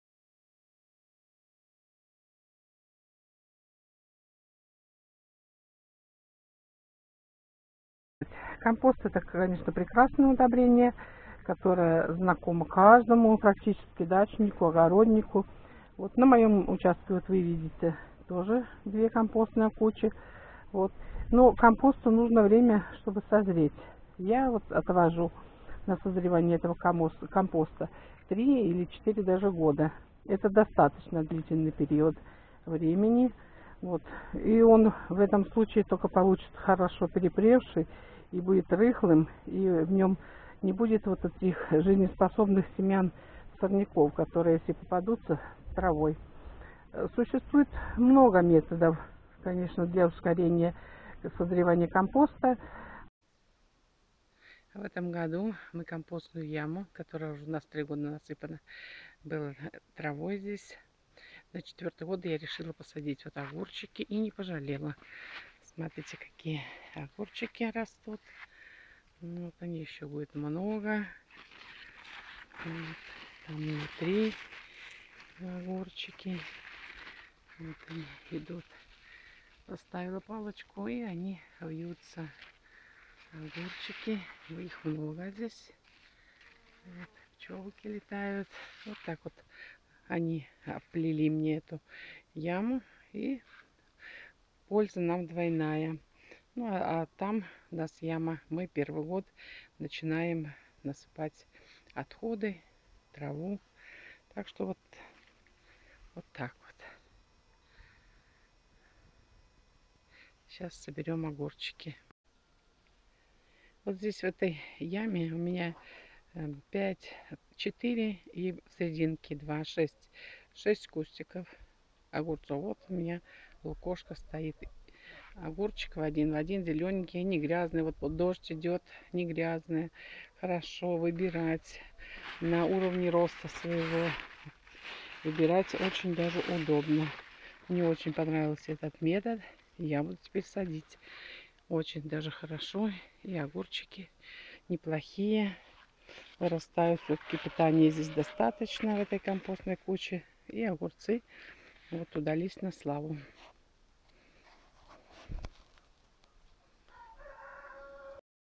КАК МОЖНО ИСПОЛЬЗОВАТЬ КОМПОСТНЫЕ ЯМЫ С ДВОЙНОЙ ПОЛЬЗОЙ НА ДАЧЕ
Компостные ямы на дачном участке это необходимое место.Отходы со стола ,траву,листву,да все,что можно мы бросаем в яму.Мы собираем три года,чтобы все хорошо перегнило и дал нам прекрасный гумус.А чтобы не бросалось в глаза окружающих можно и сделать небольшой зеленый уголок.